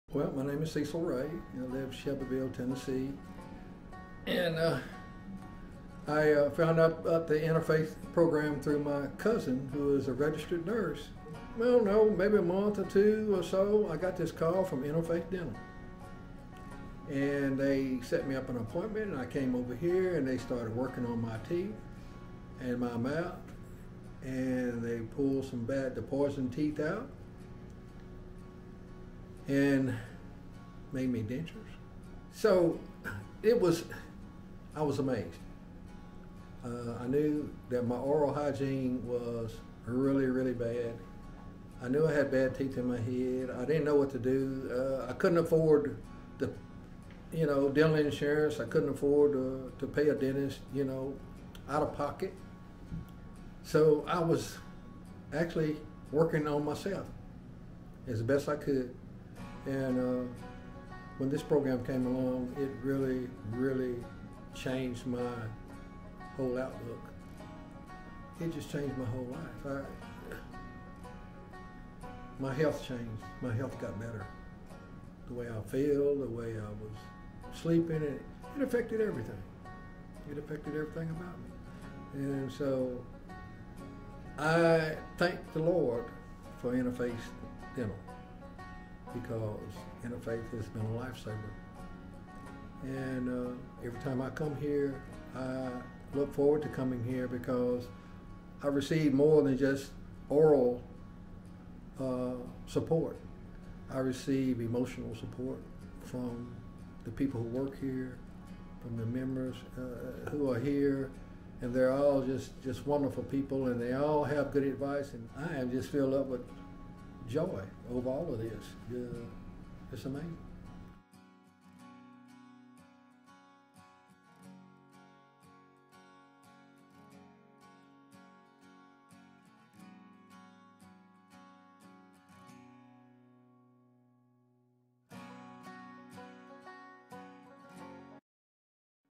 Patient Story: Cecil
Cecil has been a long patient of Interfaith Dental.  Hear his story of hope, change, and a better and healthier life.